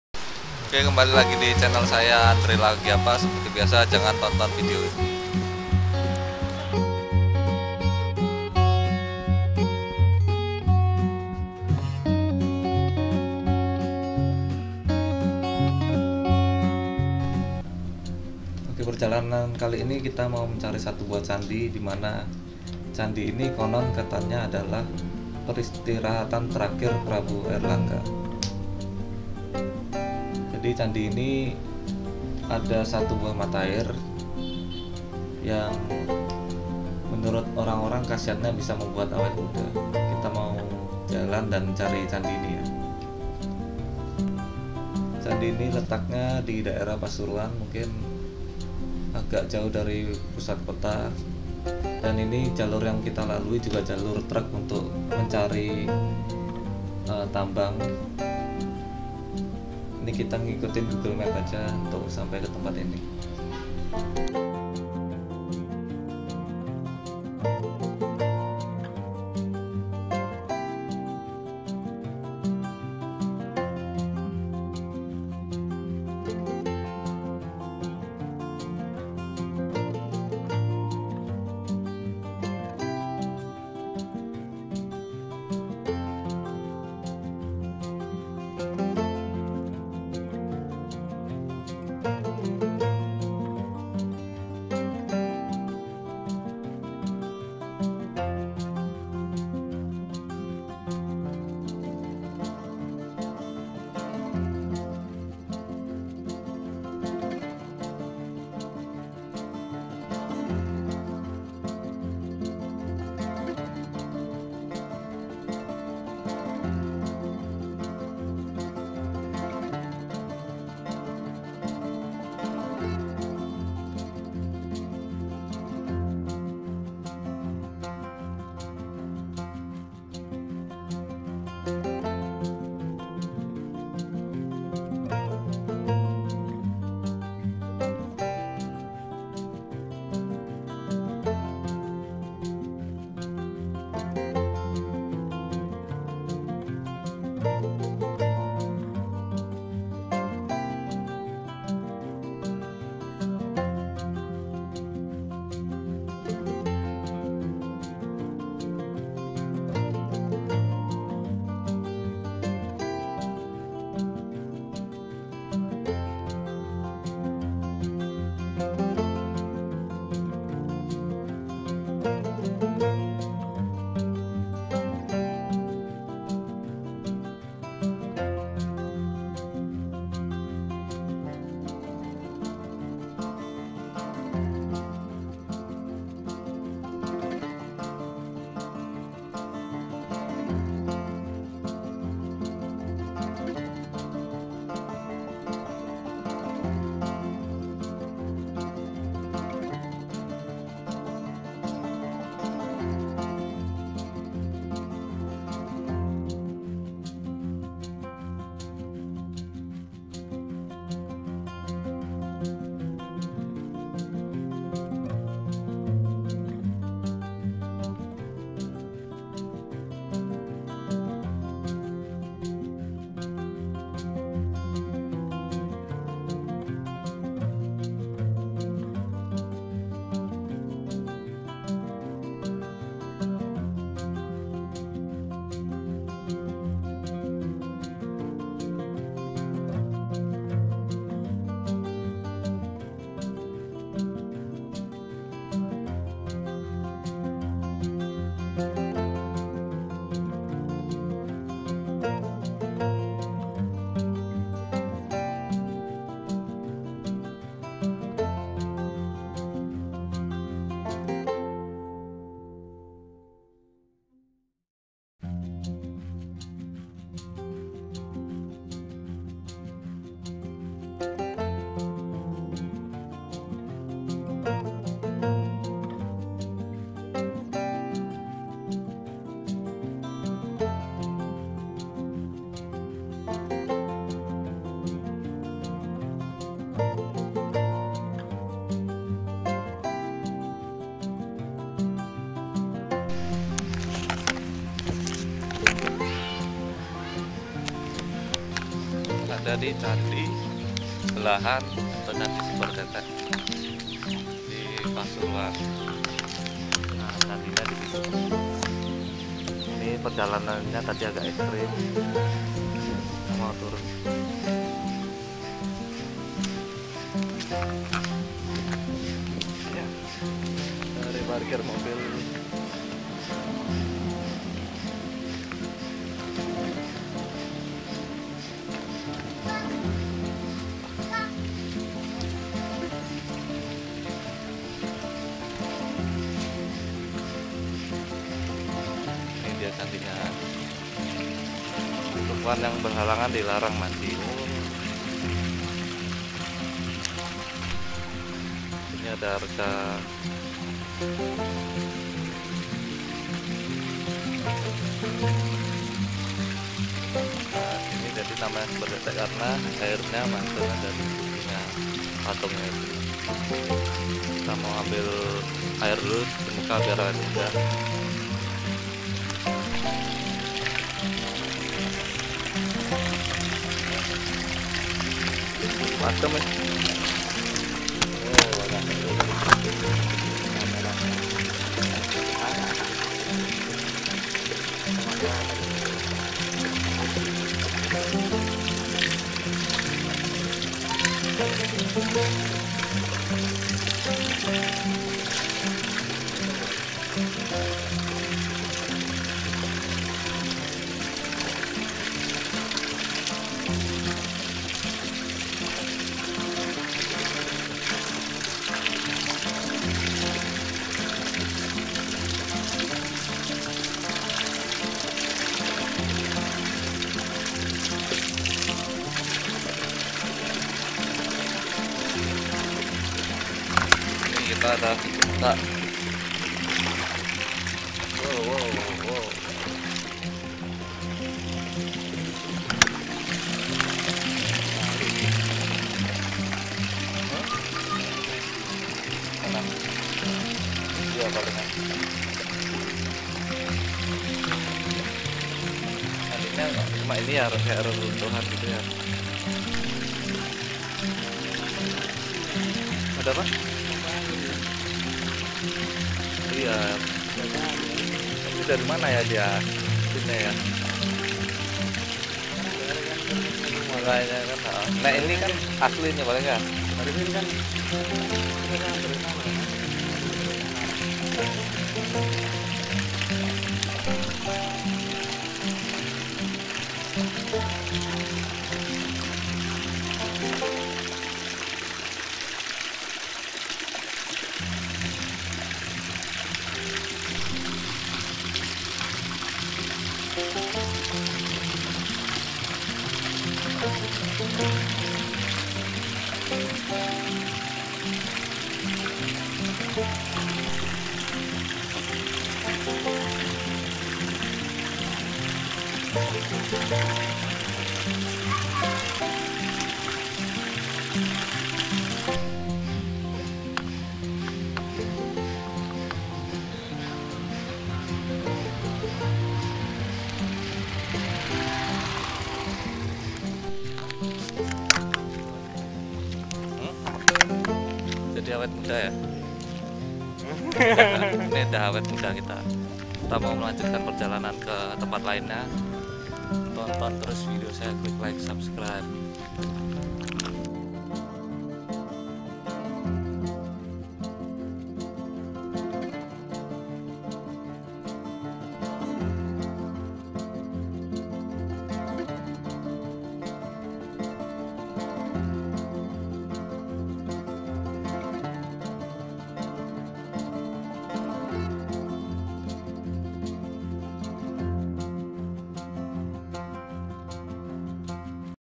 Mencari Air Awet Muda di CANDI BELAHAN PASURUAN
Mencari Air Awet Muda di CANDI BELAHAN PASURUAN. ini adalah salah satu candi terbaik yang pernah saya kunjungi. lokasi nya ada di
pasuruan. namanya adalah candin belahan pasuruan